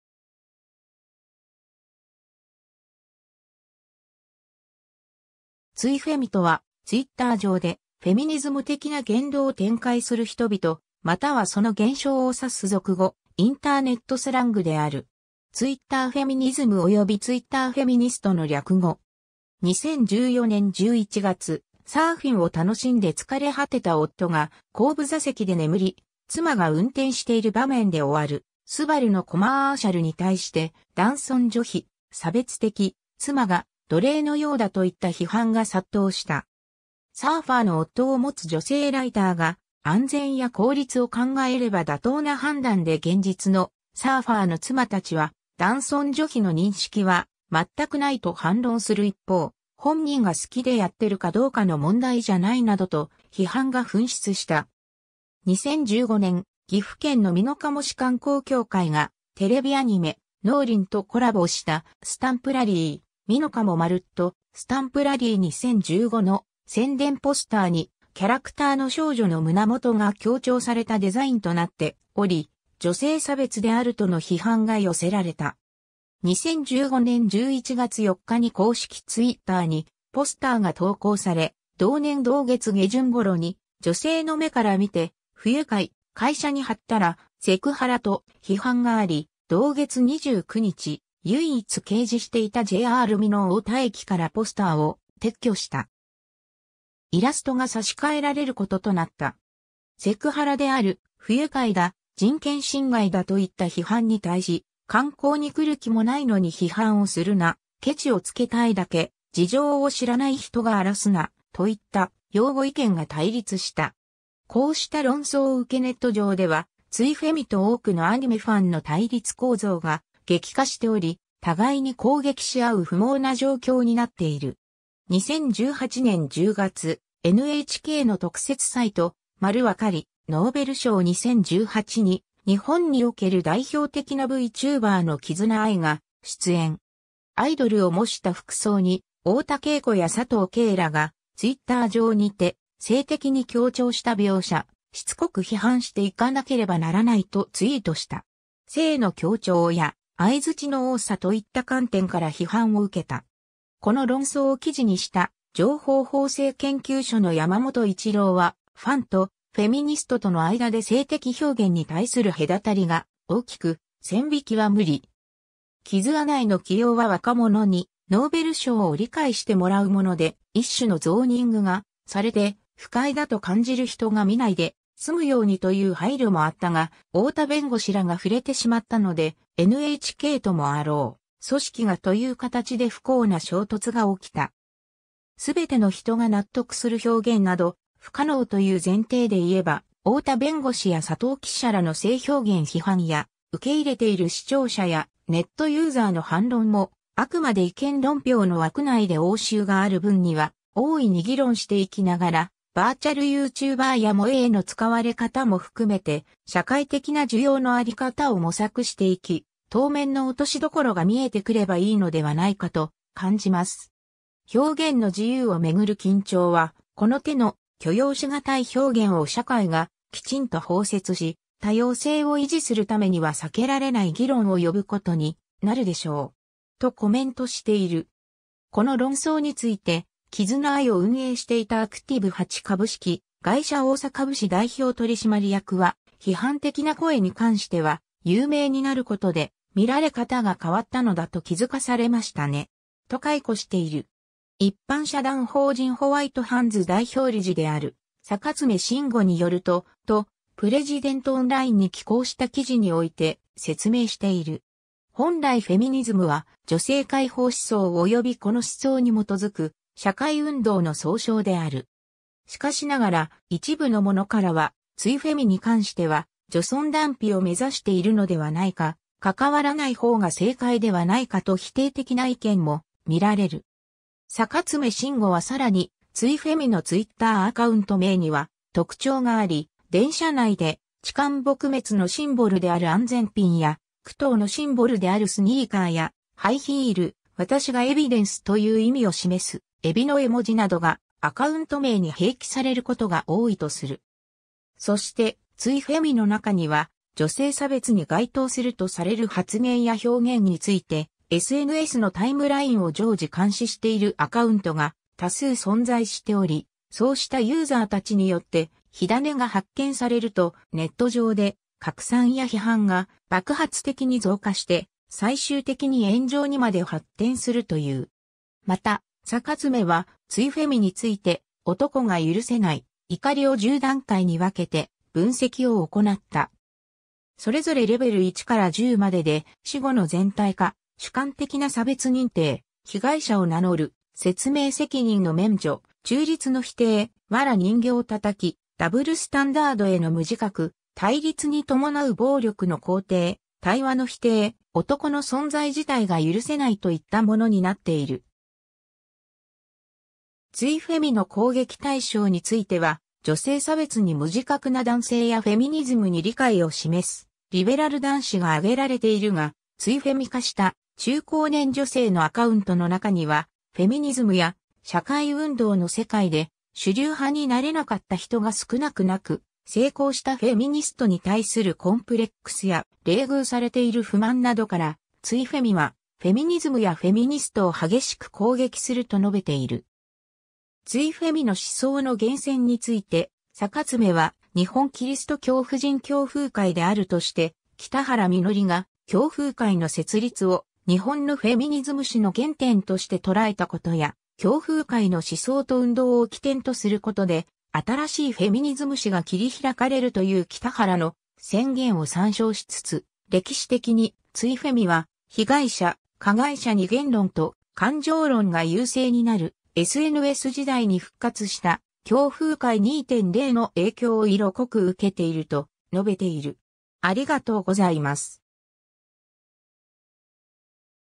ツイフェミ
ソーシャル・ネットワーキング・サービス
ラディカル・フェミニズム
ツイフェミとは、Twitter上でフェミニズム的な言動を展開する人々またはその現象を指す俗語・インターネットスラングである。
 2014年11月、サーフィンを楽しんで疲れ果てた夫が後部座席で眠り、妻が運転している場面で終わるスバルのコマーシャルに対して、男尊女卑、差別的、妻が奴隷のようだといった批判が殺到した。
サーファーの夫を持つ女性ライターが「安全や効率を考えれば妥当な判断で現実のサーファーの妻たちは男尊女卑の認識は全くない」と反論する一方、「本人が好きでやってるかどうかの問題じゃない」などと批判が噴出した。
 2015年、岐阜県の美濃加茂市観光協会がテレビアニメ「のうりん」とコラボしたスタンプラリー「みのかもまるっとスタンプラリー2015」の宣伝ポスターに、キャラクターの少女の胸元が強調されたデザインとなっており「女性差別」であるとの批判が寄せられた。
2015年（平成27年）11月4日に公式ツイッターにポスターが投稿され、同年同月下旬頃に「女性の目から見て不愉快」「会社に貼ったらセクハラ」と批判があり、同月29日、唯一掲示していたJR美濃太田駅からポスターを撤去した。
イラストが差し替えられることとなった。
セクハラである、不愉快だ、人権侵害だといった批判に対し、観光に来る気もないのに批判をするな、ケチをつけたいだけ、事情を知らない人が荒らすな、といった擁護意見が対立した。
こうした論争を受けネット上ではツイフェミと多くのアニメファンの対立構造が激化しており、互いに攻撃しあう不毛な状況になっている。
 2018年（平成30年）10月、NHKの特設サイト『まるわかりノーベル賞2018』に日本における代表的なVtuberのキズナアイが出演。
アイドルを模した服装に太田啓子や佐藤 圭らがTwitter上にて「性的に強調した描写」「しつこく批判していかなければならない」とツイートした。
「性の強調」や「相槌の多さ」といった観点から批判を受けた。
この論争を記事にした情報法制研究所の山本一郎は 『ファンとフェミニストとの間で性的表現に対する隔たりが大きく線引きは無理。
キズアナイの起用は若者にノーベル賞を理解してもらうもので、一種のゾーニングがされて、不快だと感じる人が見ないで済むようにという配慮もあったが、太田弁護士らが触れてしまったので「NHKともあろう組織が」という形で不幸な衝突が起きた。
全ての人が納得する表現など不可能という前提で言えば、太田弁護士や佐藤記者らの性表現批判や受け入れている視聴者やネットユーザーの反論も、あくまで意見論評の枠内で応酬がある分にはおおいに議論していきながら、バーチャルユーチューバーや萌え絵の使われ方も含めて社会的な受容の在り方を模索していき、当面の落としどころが見えてくればいいのではないかと感じます。
表現の自由を巡る緊張は、この手の「許容しがたい表現を社会がきちんと包摂し、多様性を維持するためには避けられない議論」を呼ぶことになるでしょう。
』（要約）とコメントしている。
 この論争について、キズナアイを運営していたActiv8株式会社大坂武史代表取締役は「批判的な声に関しては、有名になることで「見られ方が変わったのだ」と気づかされましたね。
」と回顧している。
 一般社団法人ホワイトハンズ代表理事である坂爪真吾によると、 と、プレジデントオンラインに寄稿した記事において説明している。
 本来フェミニズムは、女性解放思想およびこの思想に基づく社会運動の総称である。
しかしながら、一部の者からはツイフェミに関しては「女尊男卑を目指しているのではないか」「関わらないほうが正解ではないか」と否定的な意見も見られる。
坂爪真吾はさらに、ツイフェミのツイッターアカウント名には特徴があり、電車内で痴漢撲滅のシンボルである安全ピンや、KuTooのシンボルであるスニーカーやハイヒール、「私がエビデンス」という意味を示す海老（エビデンスとかけている）の絵文字などがアカウント名に併記されることが多いとする。
そして、ツイフェミの中には女性差別に該当するとされる発言や表現についてSNSのタイムラインを常時監視しているアカウントが多数存在しており、そうした...